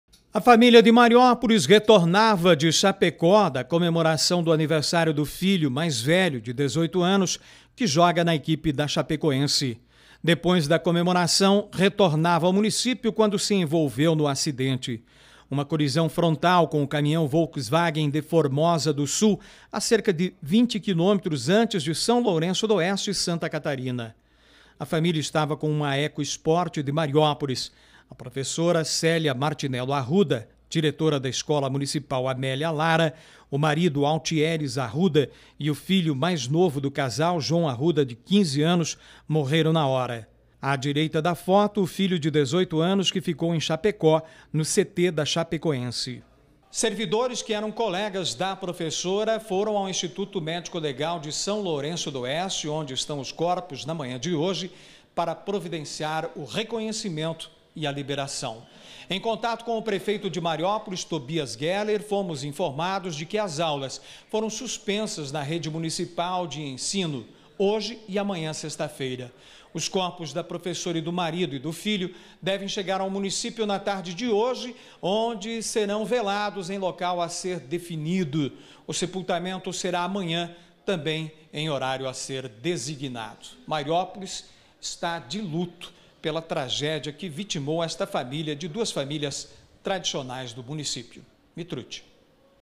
Família de Mariópolis morre em acidente retornando de Chapecó
Eles foram comemorar o aniversário de um dos filhos que joga na Chapecoense.